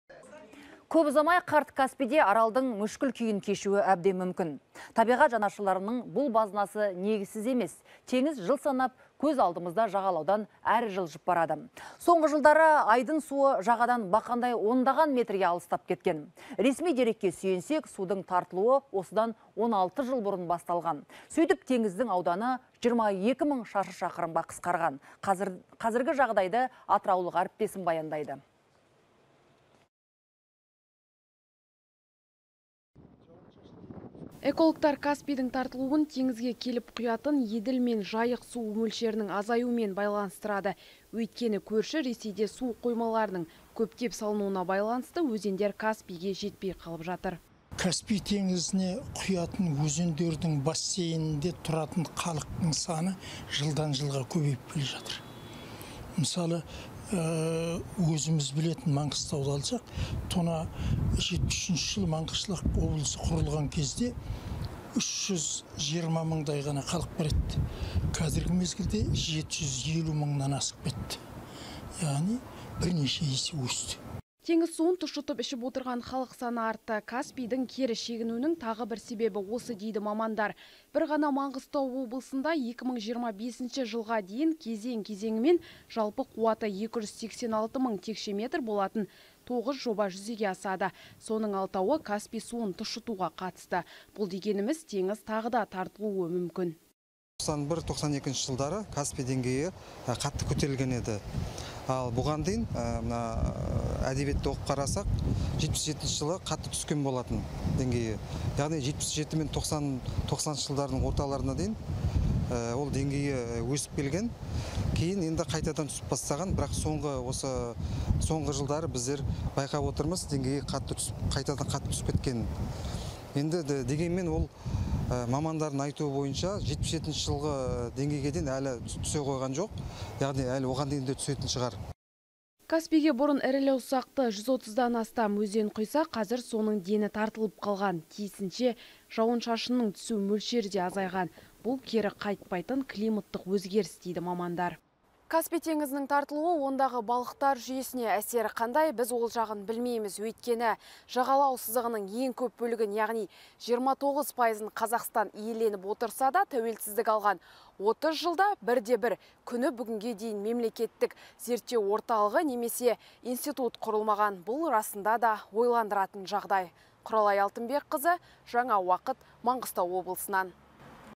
Каспий Аралдың күйін кешуі мүмкін
Табиғат жанашырлары Каспийге алаңдаушылық білдірді.